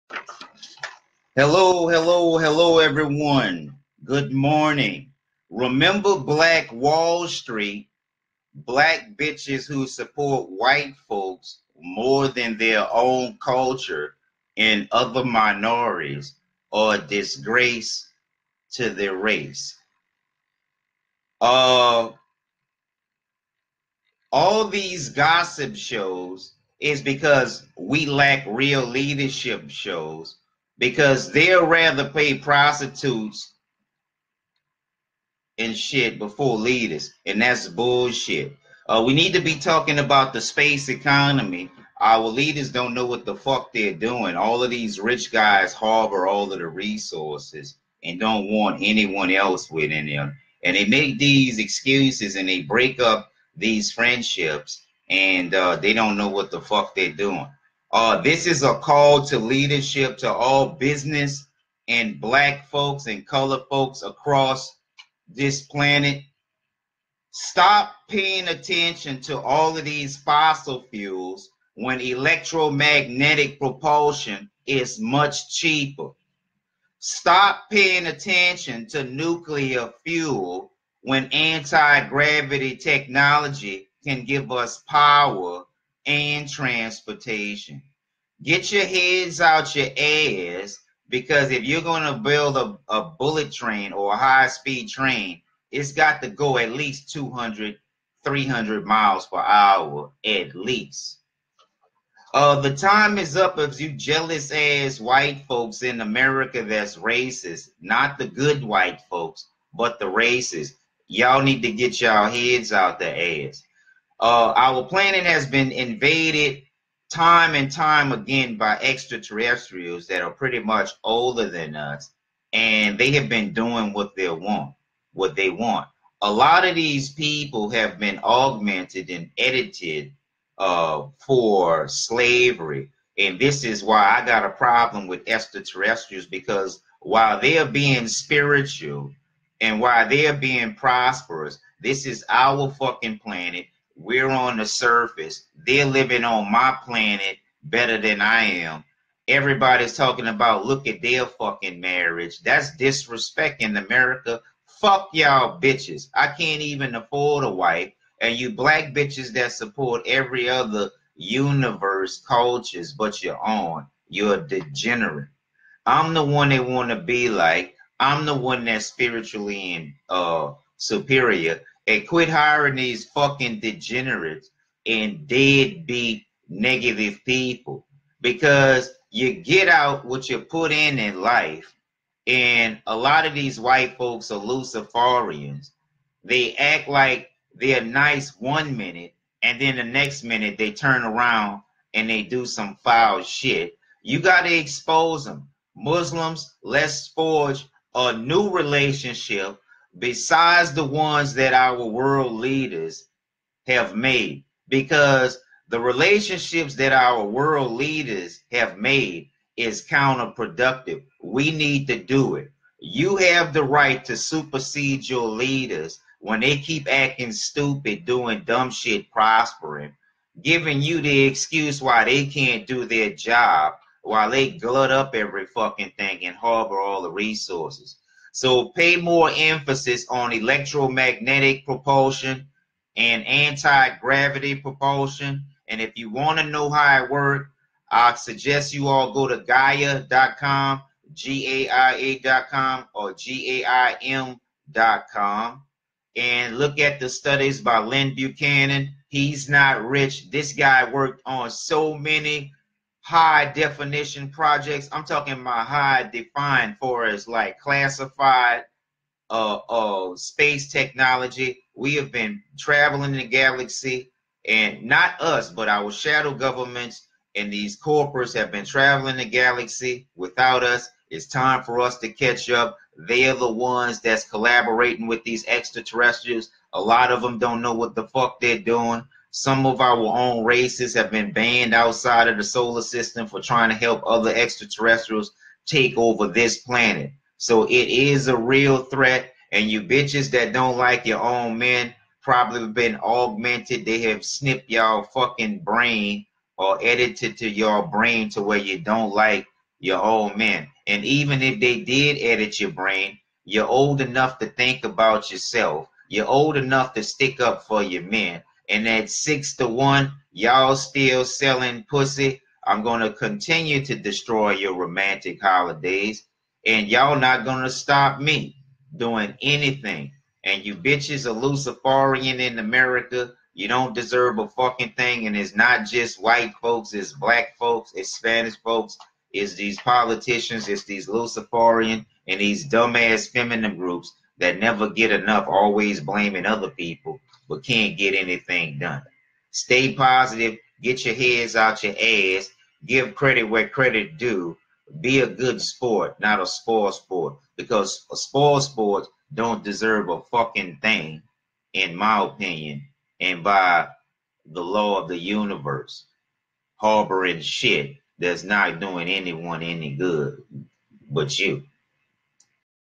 Pass the pain to appropriate people; The Universe favors us instead
Grassroots Community Activities means when leaders are acting stupid and prosper you have a right to supersede them. This does not mean over-throw but set better examples in leadership arena, and if needed we must replace them with better leaders.